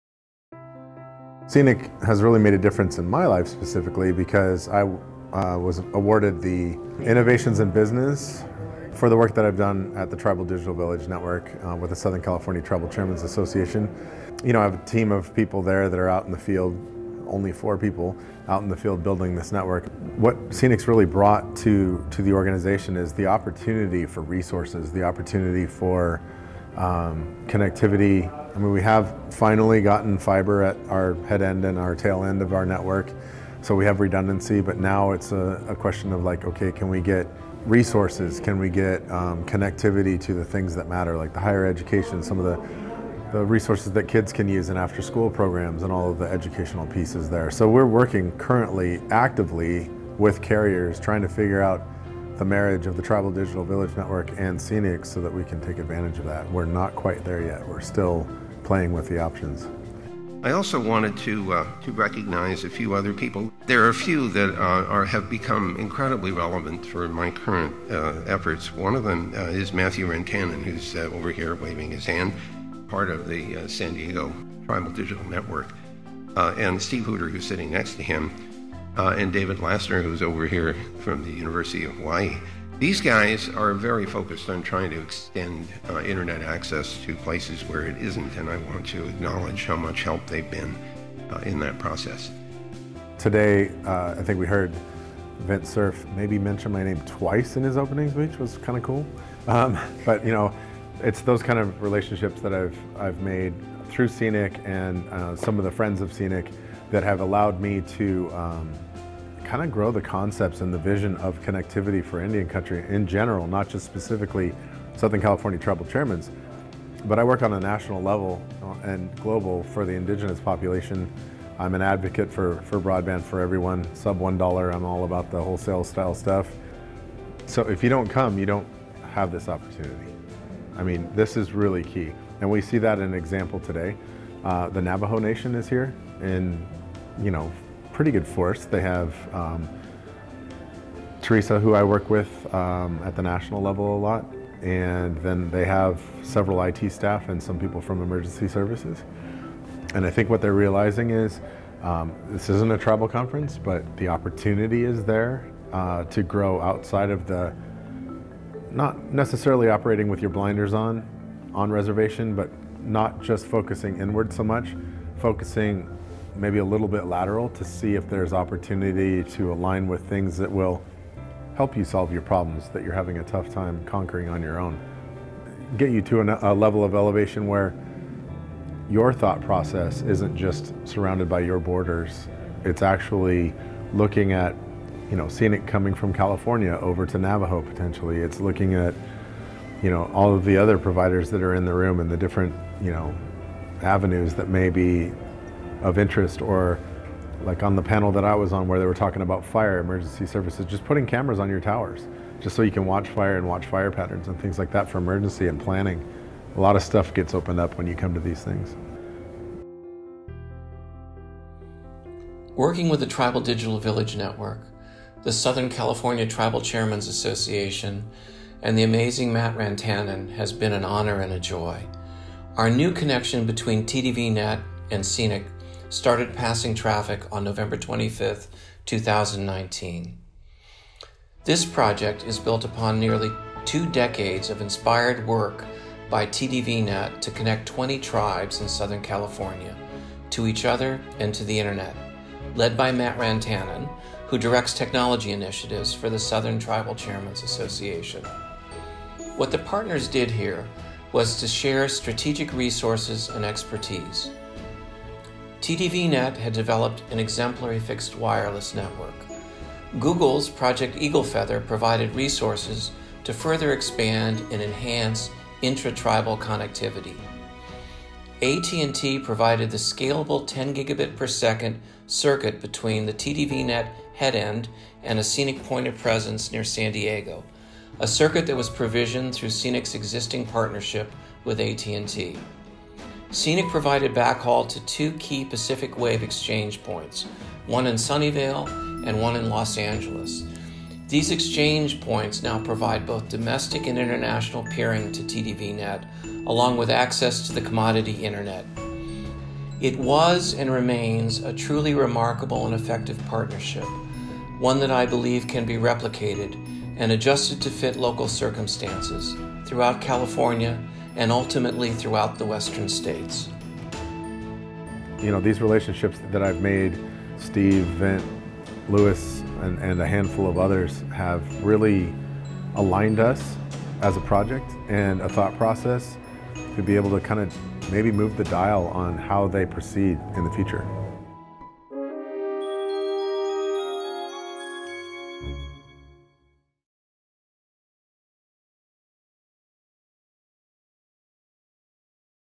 Matthew Rantanen and the CENIC Connection
Matt Rantanen speaks of the partnership between the Tribal Digital Village Network (TDVNet) and the Corporation for Education Network Initiatives in California (CENIC). Vint Cerf and Louis Fox add to the conversation of networking beyond cities and into rural broadband communities.